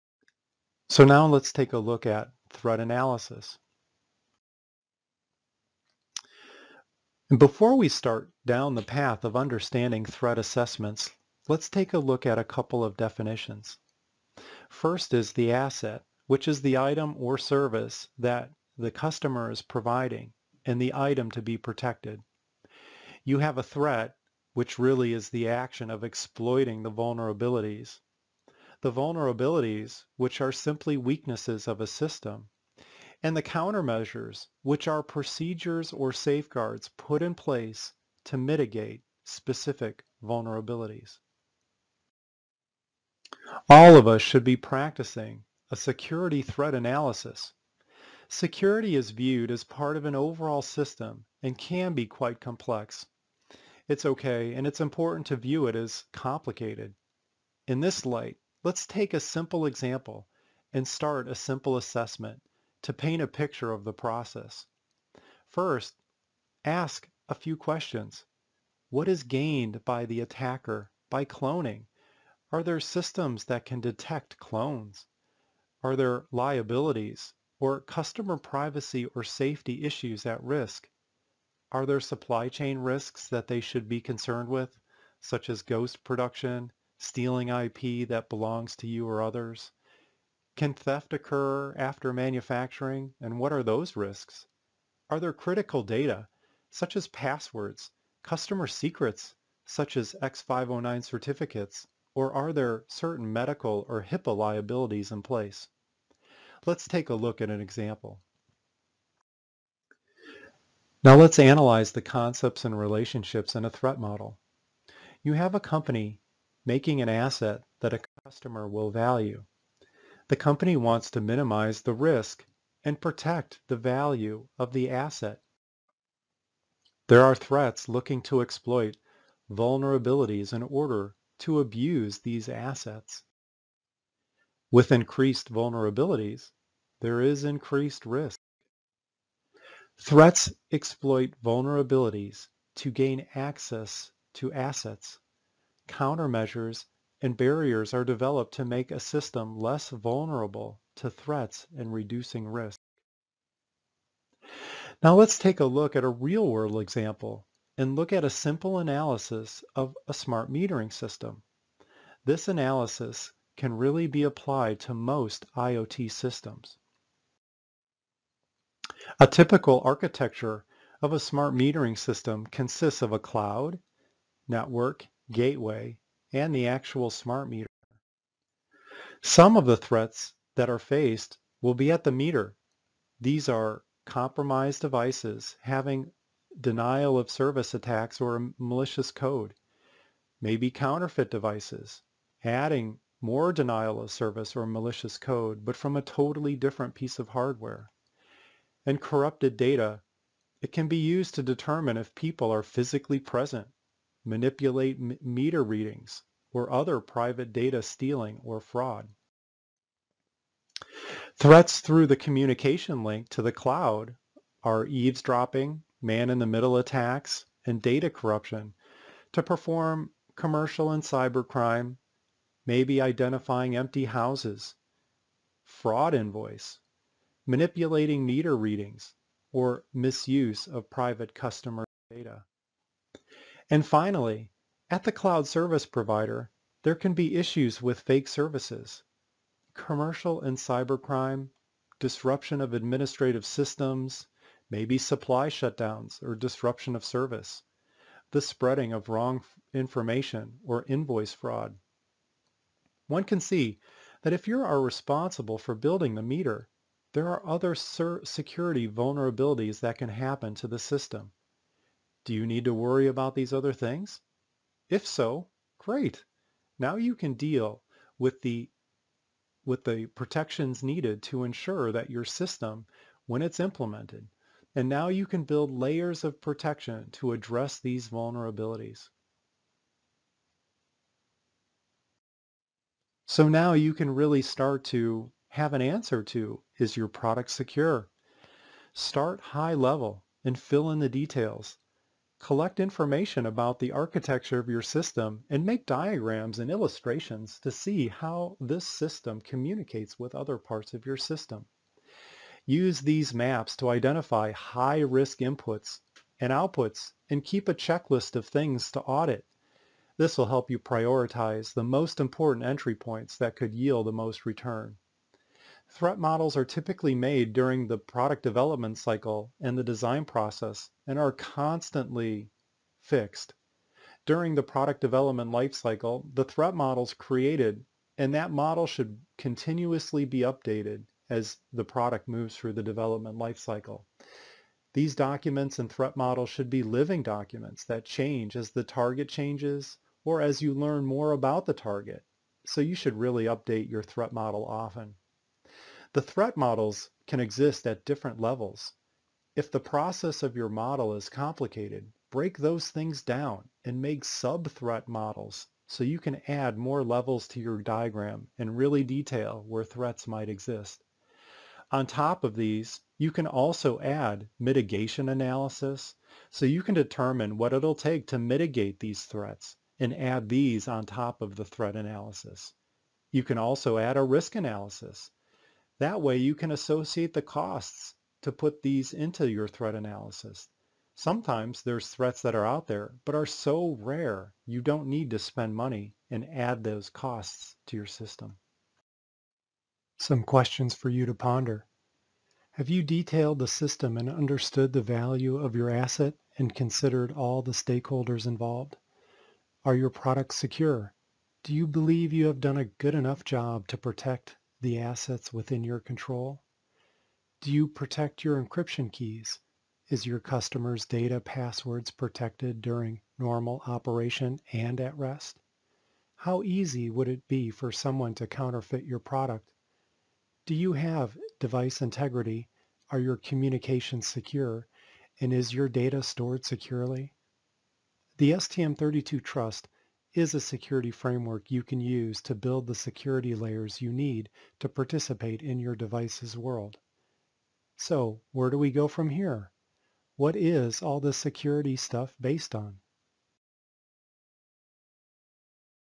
Security Part5 - How to define your security needs - 2 Threat Analysis
Learn  why security is important and how STM32Trust framework provide ways to mitigate the threats and vulnerabilities of your applications
Get familiar with today main security risks within embedded systems and find out how STM32Trust framework  could help you to better secure  your assets .

• Engineers looking for basic information about security threat analysis
• Engineers interested in security topics within nowadays embedded systems

•  Better understanding of threats analysis with practical examples
•  overview of root of trust and its basic building blocks to establish a chain of trust